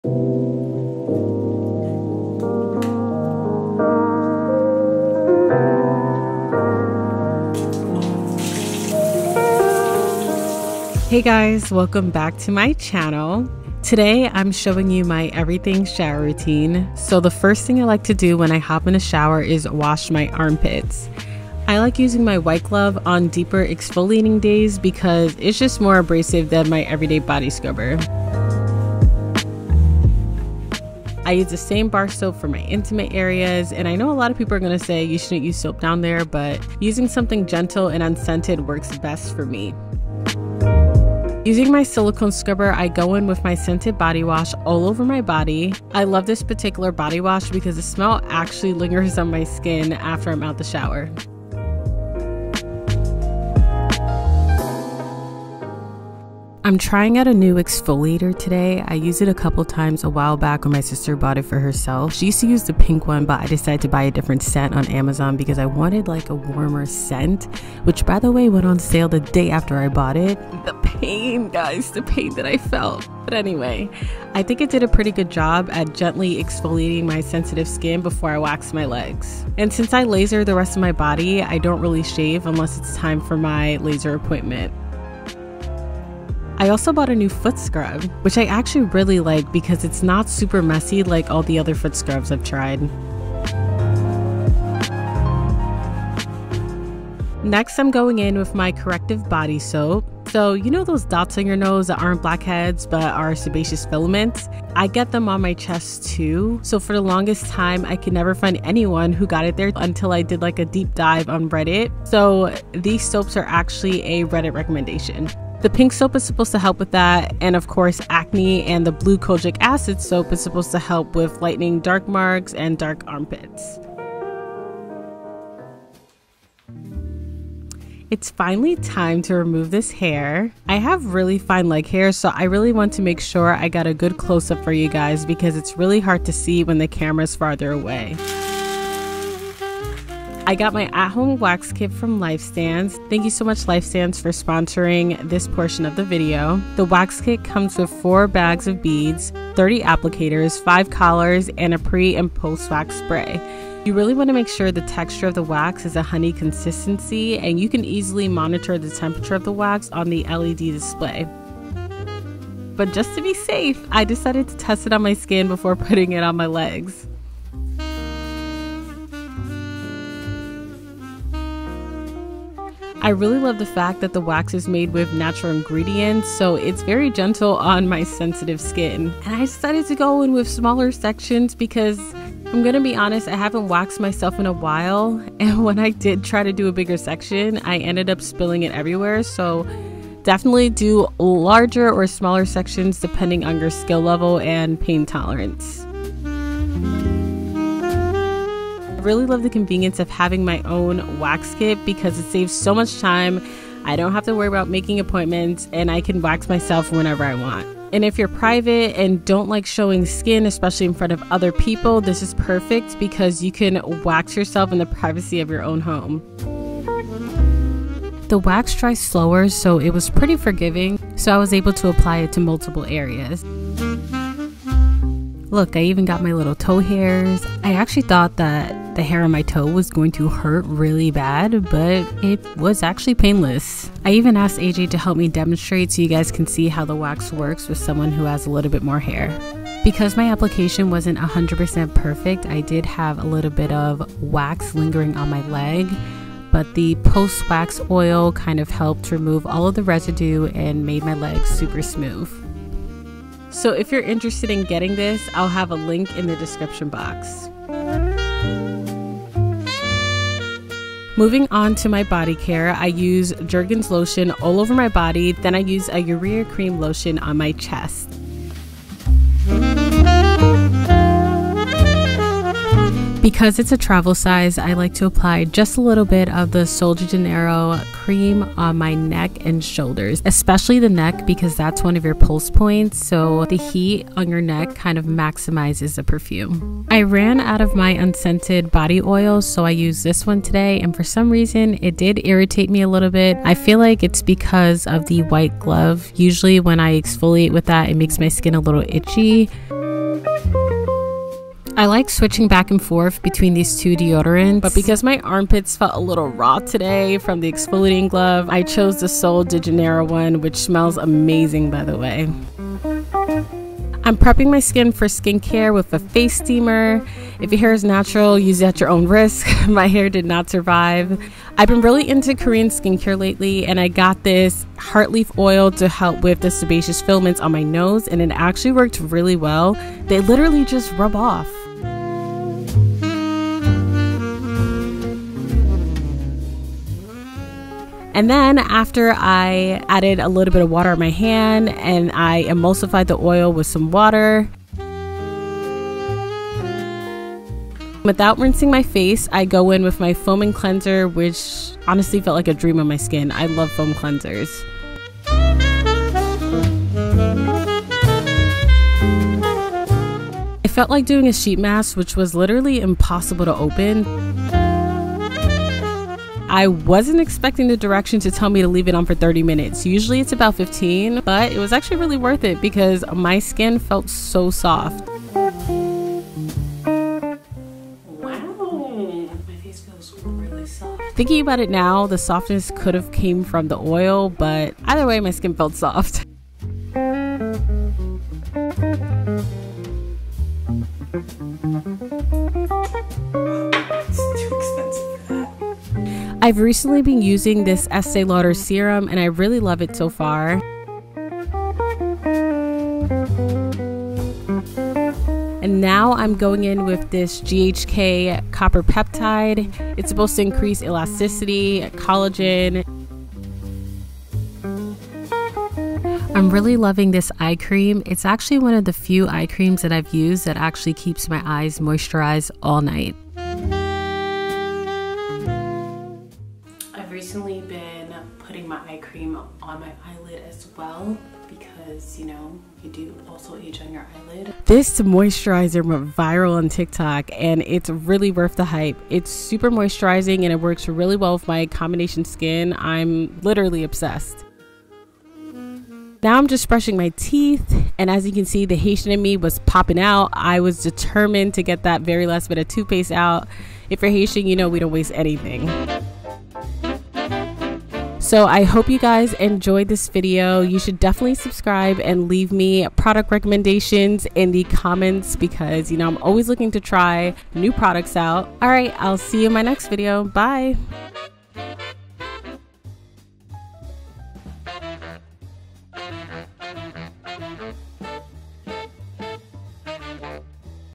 EVERYTHING SHOWER ROUTINE | SMELL GOOD ALL DAY!
Hi everyone! I hope you enjoyed seeing my Everything Shower Routine! This shower routine makes me smell good all day and gives me the softest skin EVERRR! Here’s the link to the @lifestancewax wax kit

Link to all of the products I used